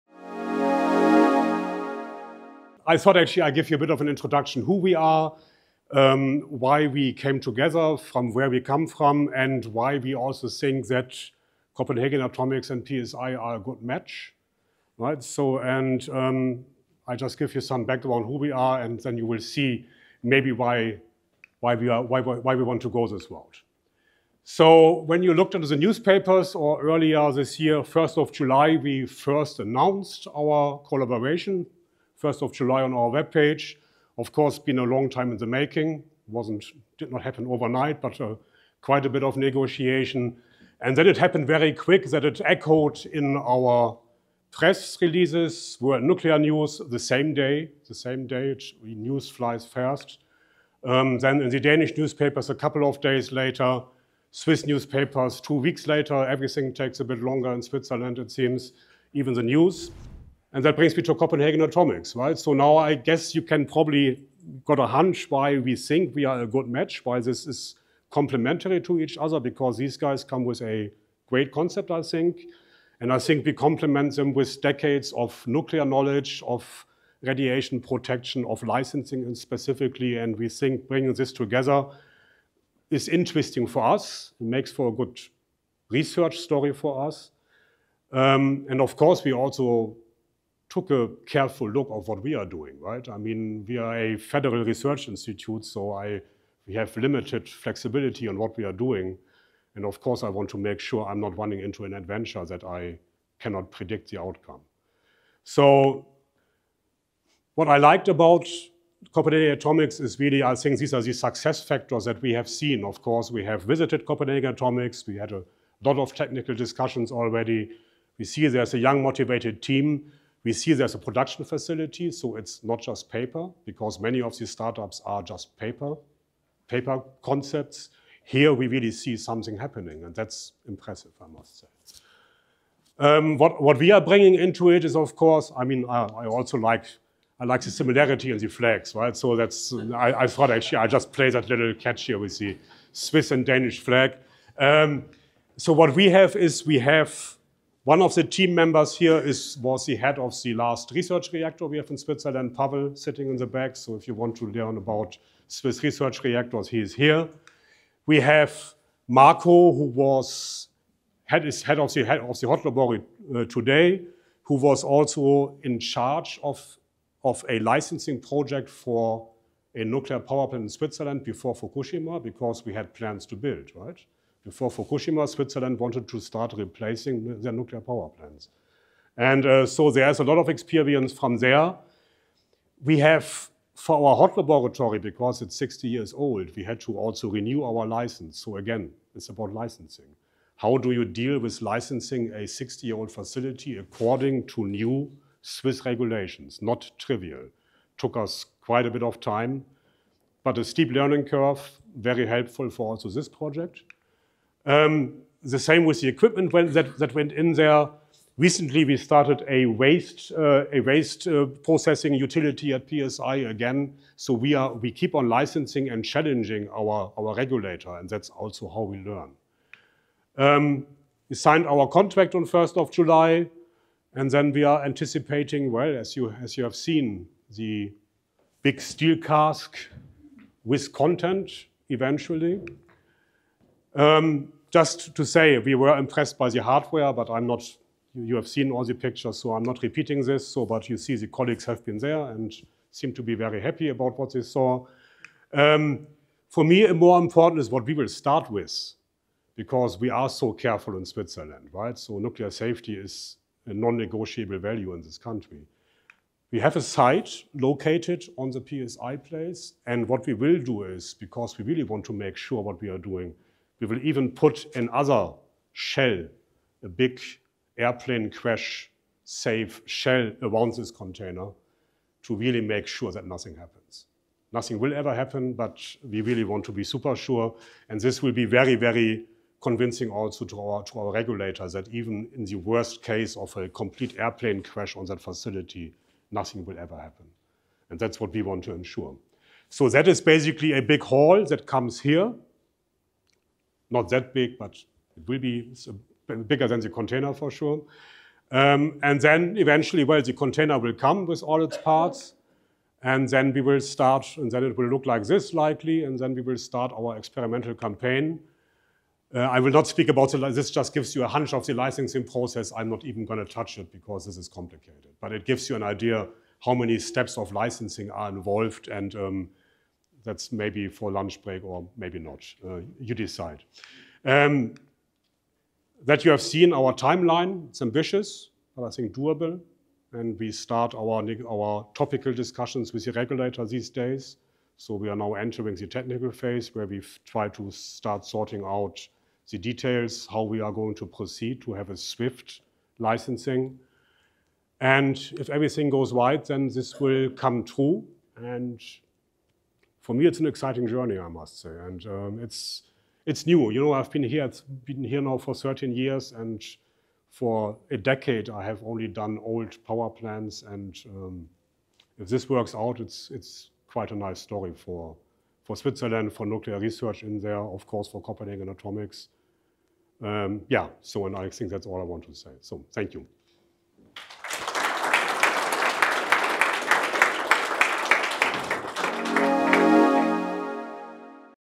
Nuclear expert explains the secret to Thorium energy (short version)
Dive into the highlights of the Switzerland event, where Paul Scherrer Institute's Andreas Pautz discusses the groundbreaking molten salt experiment in collaboration with Copenhagen Atomics. Learn more about this exciting step in nuclear innovation!